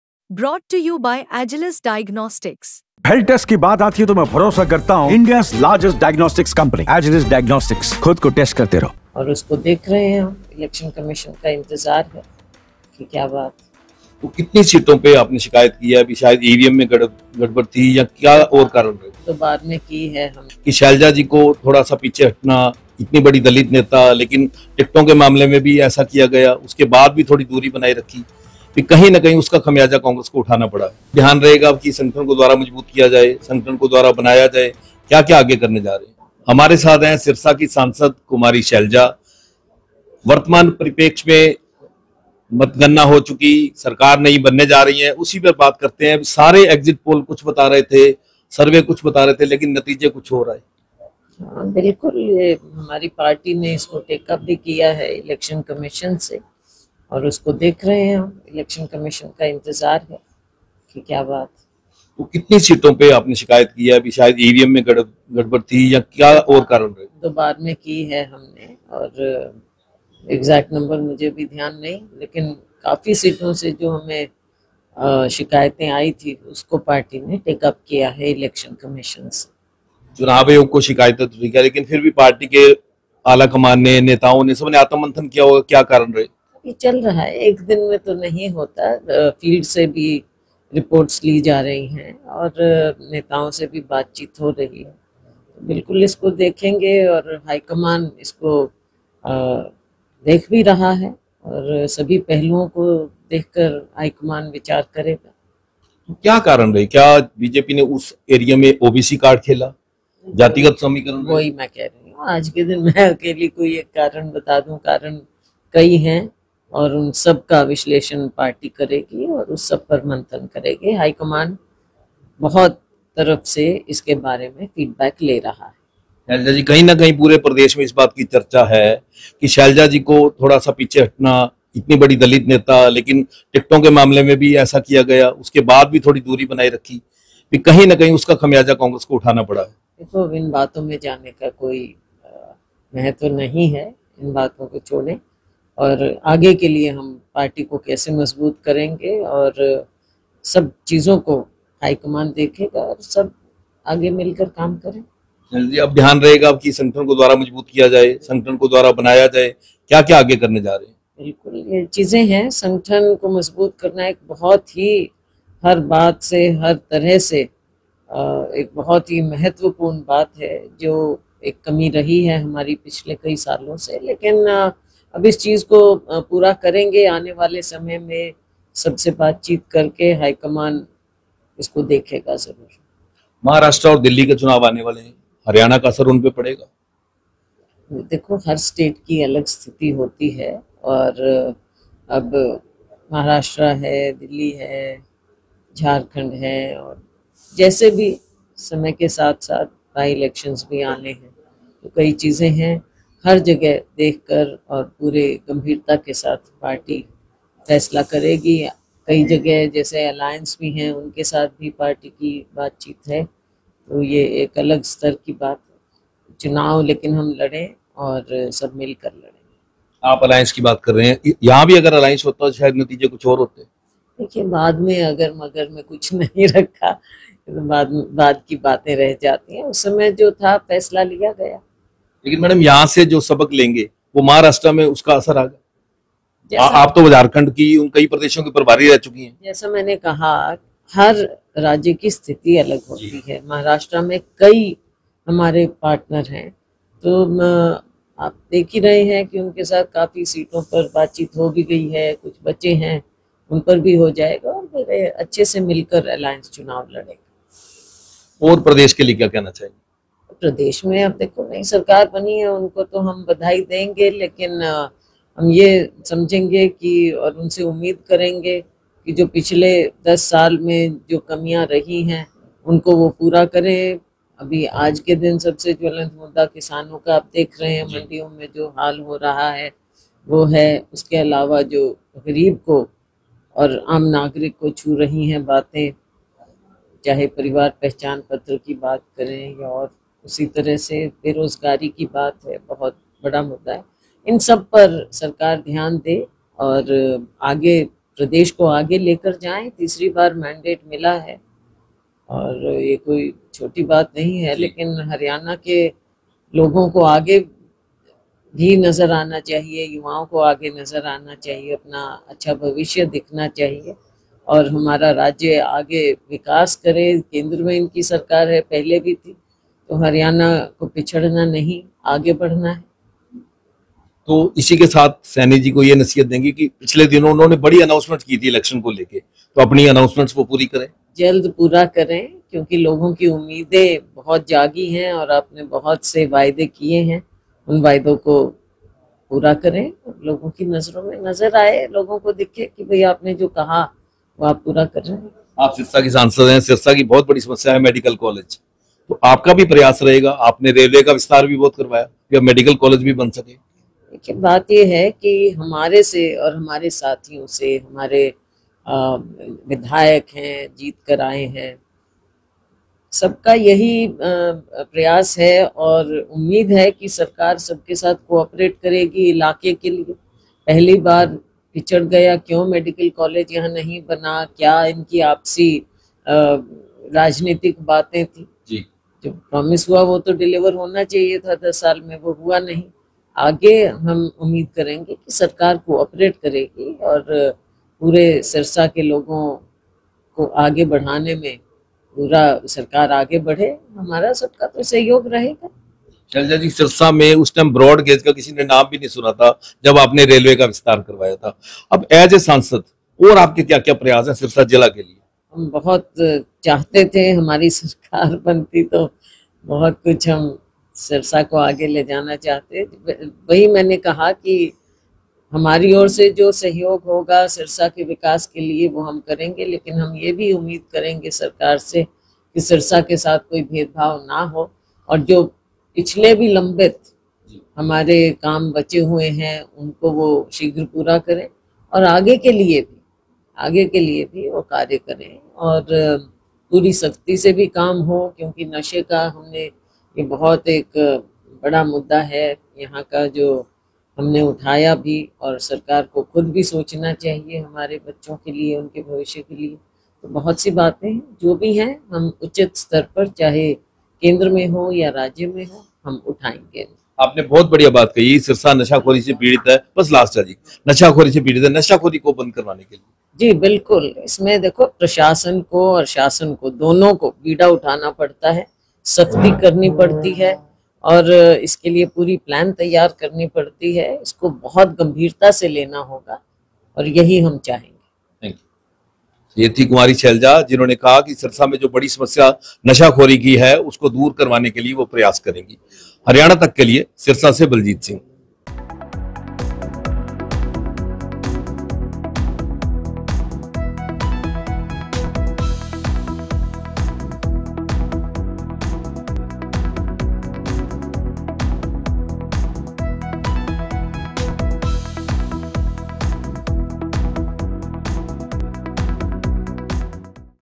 Deepak Babria के इस्तीफे की पेशकश,Selja ने पहली बार गिनाई हार की वजह! 20 सीटों पर दोबारा गिनती होगी?
EXCLUSIVE: Deepak Babria के इस्तीफे की पेशकश के बाद Kumari Selja ने कांग्रेस के आगे के प्लान पर क्या कहा? हार की वजहों पर दिए सारे सवालों के जवाब!